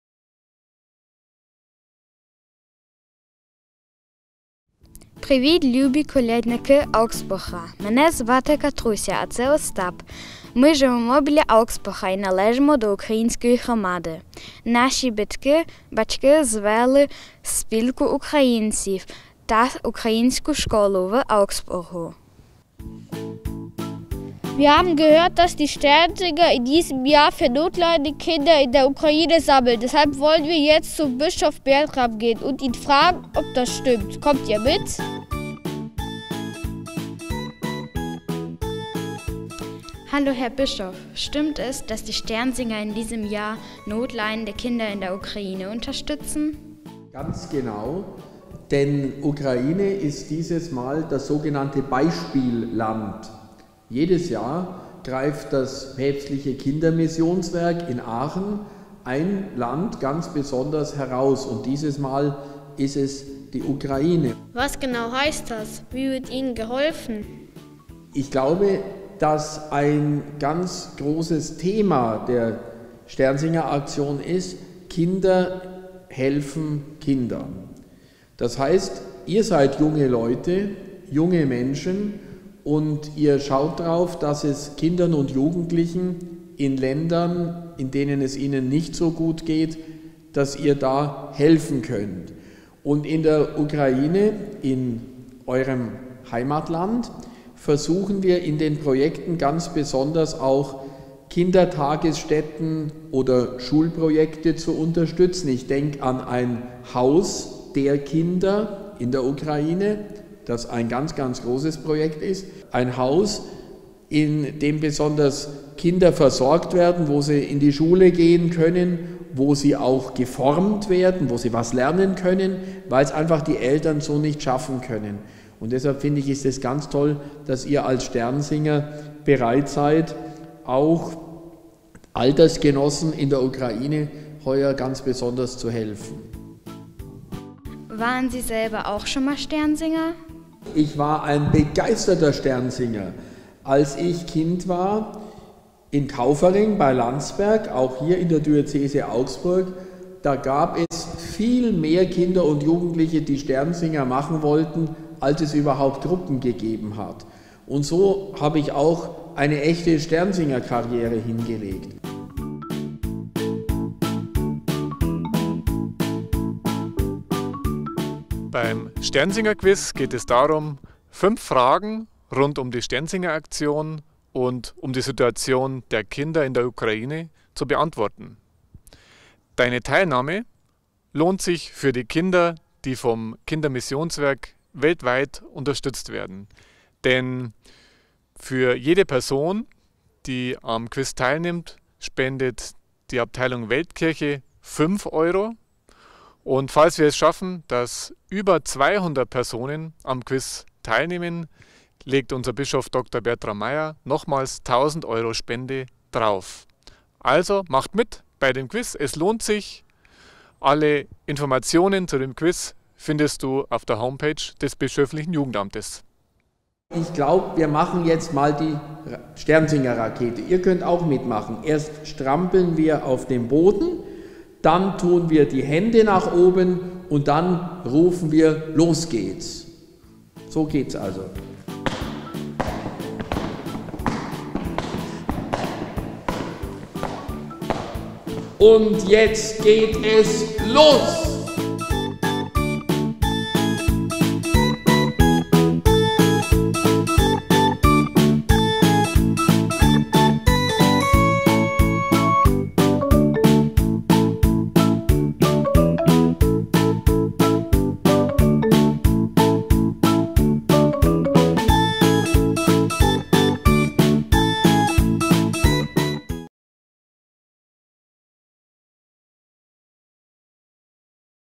dietoreweit - Aktionsfilm "Sternsinger: Heller denn je"
„Sternsingen – Heller denn je!“: So lautet die Botschaft der diesjährigen Dreikönigsaktion, zu der Bischof Dr. Bertram Meier alle Sternsinger*innen aufruft.
Und auf diese wartet heuer eine ganz besondere Aktion: Ein Sternsinger-Quiz!

Tolle Ideen und Tipps für kontaktloses Sternsingen findet ihr auf der Website der Sternsinger.

Merkt euch auf jeden Fall den Eröffnungsgottesdienst im Livestream: am 29.